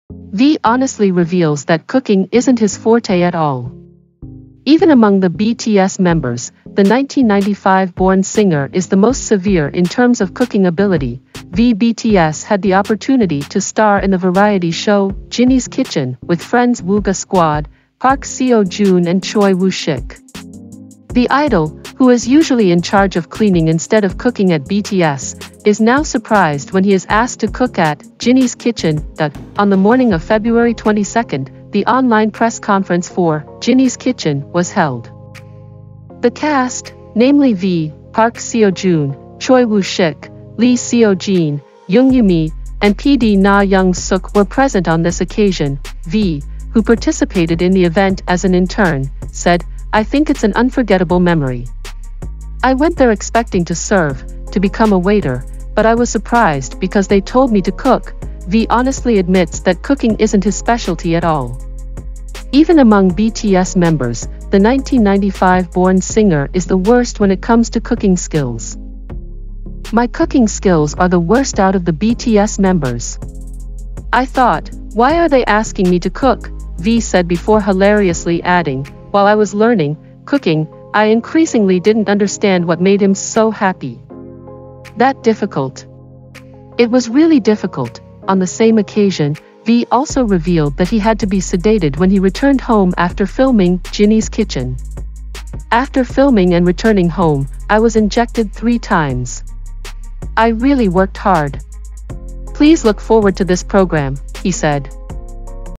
V BTS IS VERY HONEST IN REVEALING THINGS THAT ARE NOT HIS EXPERTISE
KPOP TV - V Honestly Reveals That Cooking Isn't His Forte At All. Even among the BTS members, the 1995-born singer is the most severe in terms of cooking ability.